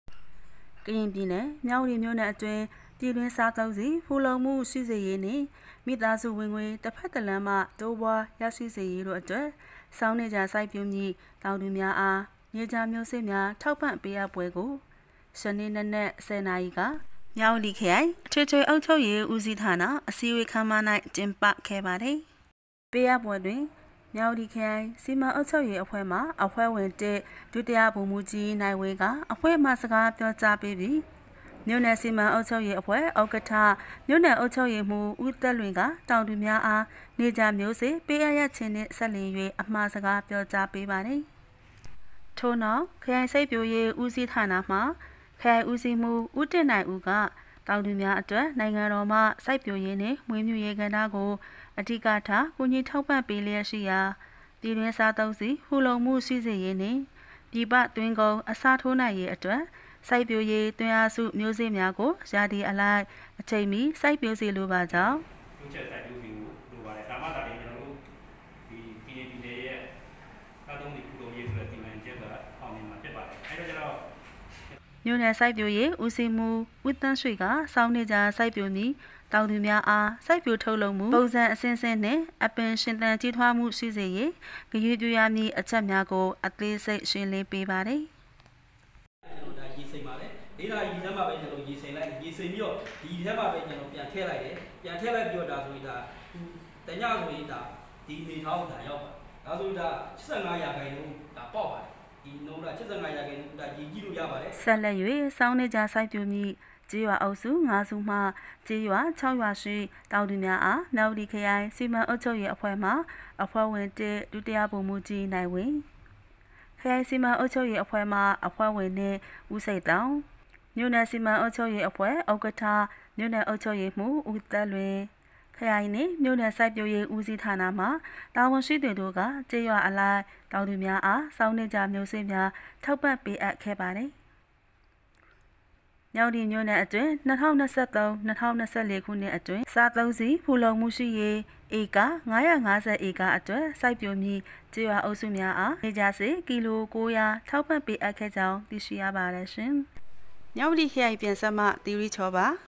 မြဝတီမြို့၌ ဆောင်းနေကြာ စိုက်ပျိုးမည့် တောင်သူများအား နေကြာမျိုးစေ့များ ထောက်ပံ့ပေးအပ်
မြဝတီ စက်တင်ဘာ ၂၁
သီရိချော